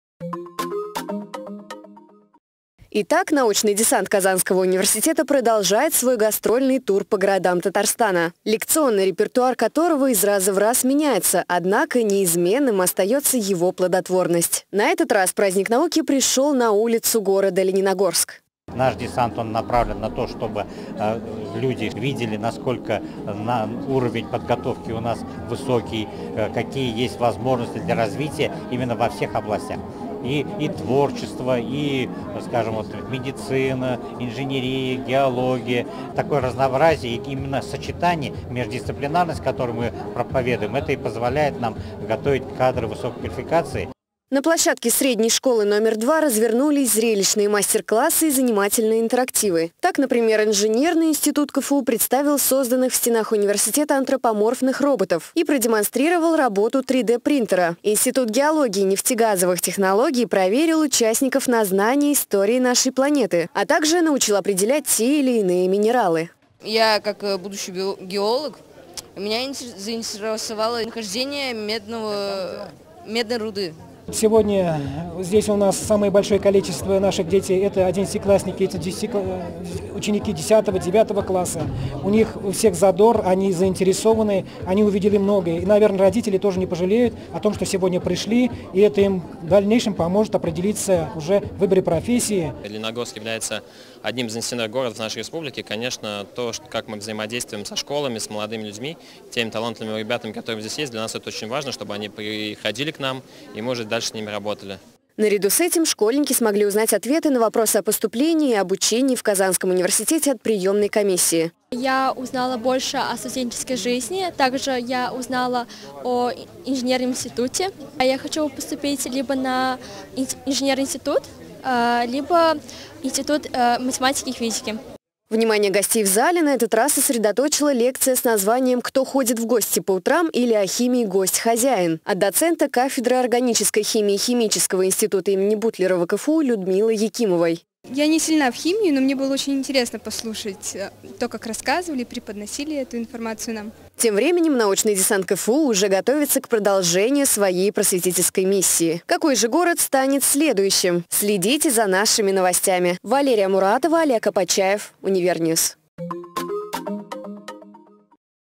ВЫСАДКА НАУЧНОГО ДЕСАНТА КФУ В ЛЕНИНОГОРСКЕ
Масштабная просветительская акция Казанского университета "Научный десант КФУ" продолжает свою работу! Какой город стал следующим пунктом посещения команды ВУЗа?